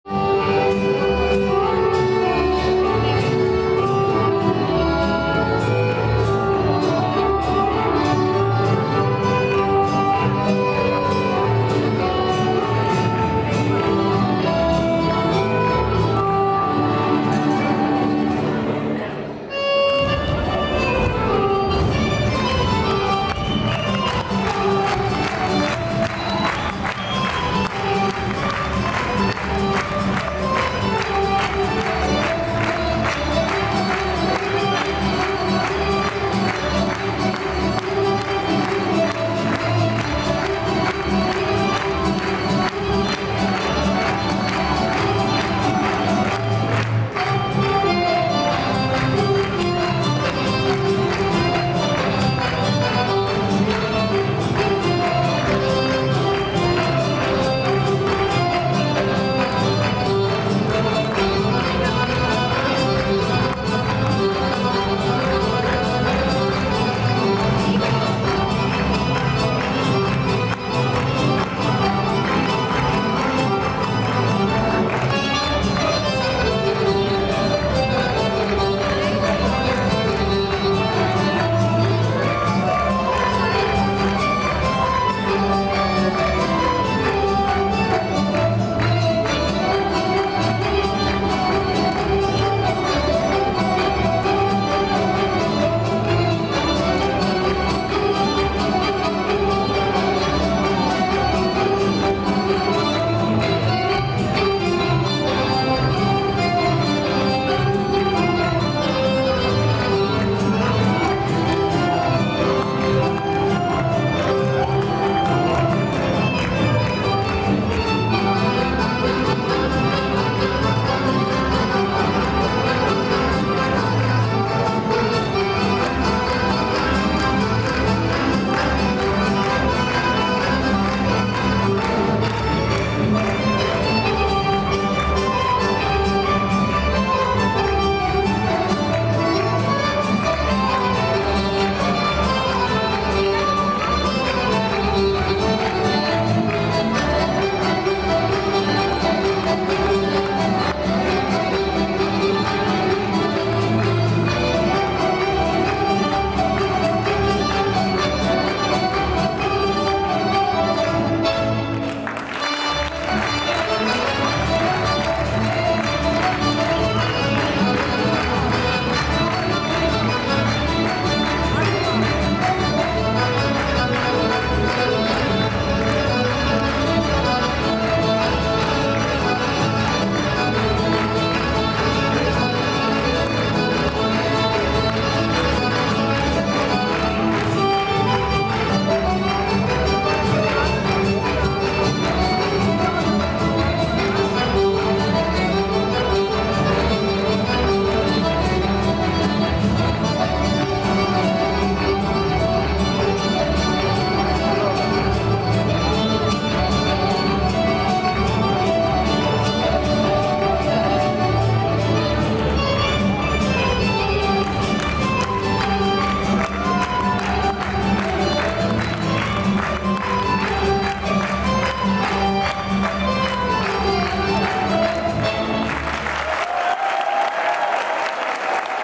OKUD "Milomir Petrovic" - Vojvodjanske igre iz Banata, III ansambl 15.04.2017.
Godisnji koncert OKUD-a "Milomir Petrovic" iz Sremcice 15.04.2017.
Izvodi: III ansambl
Umetnicki rukovodilac: Ana Mladenovic
SRC Pionirski grad